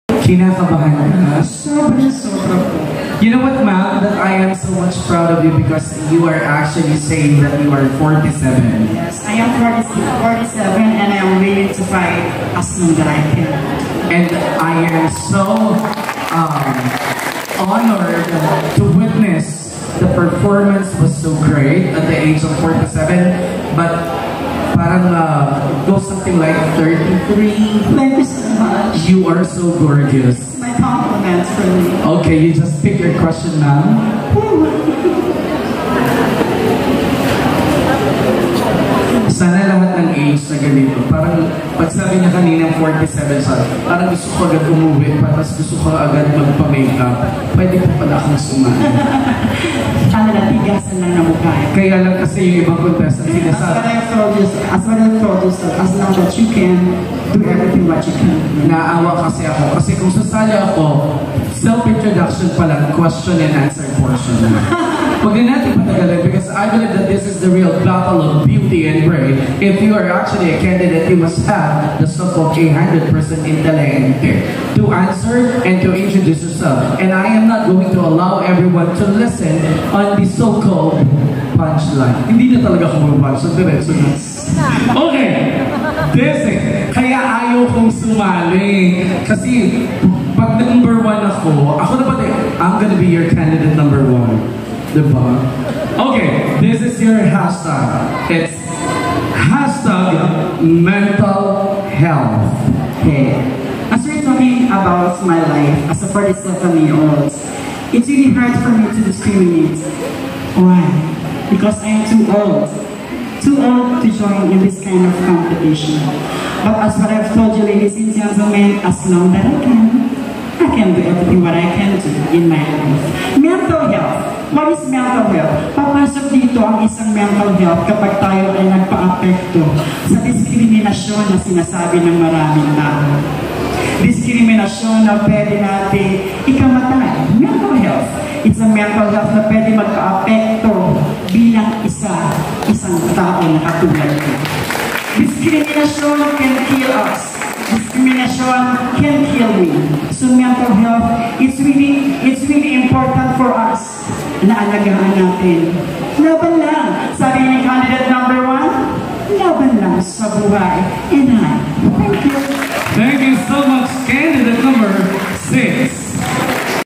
Ms. Chatt Dawi's Answer that Pushes her to top 5 finalist I Ms Gay Parang 2024.
she is a 47 years old contesera. despite her being the oldest, she manages to land in top 5 finalist and ultimately won her the 4th runner up.

this is real courage and determination.

as she said, LABAN LANG.